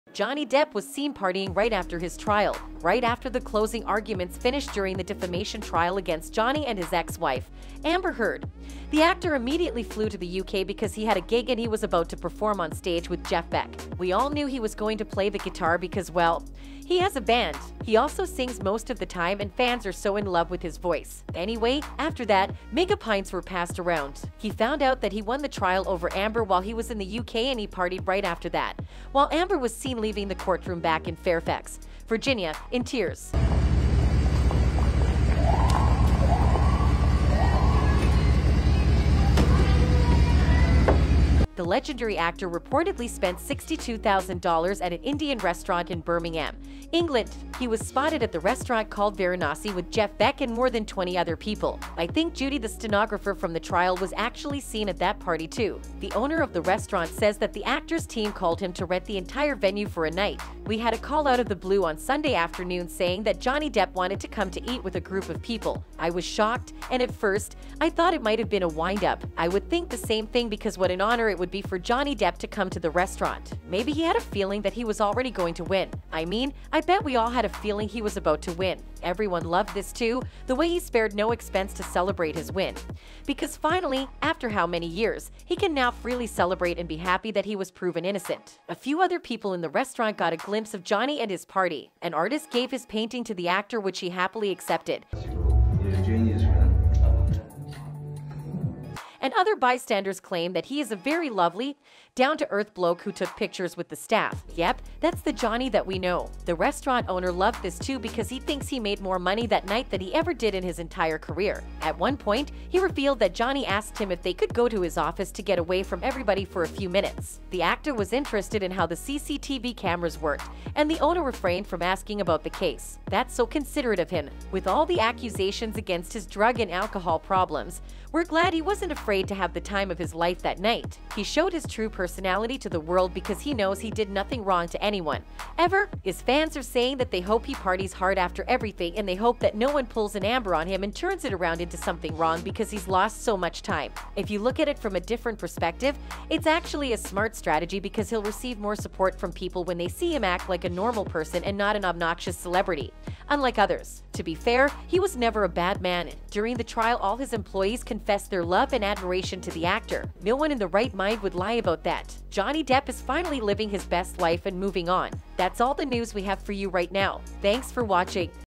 Johnny Depp WIN! His Genius Post Trial Strategy SAVED His Career!
Johnny Depp was seen partying right after his trial.

Right after the closing arguments finished during the defamation trial against Johnny and his ex-wife, Amber Heard, the actor immediately flew to the UK because he had a gig, and he was about to perform on stage with Jeff Beck.

We all knew he was going to play the guitar because, well, he has a band. He also sings most of the time and fans are so in love with his voice.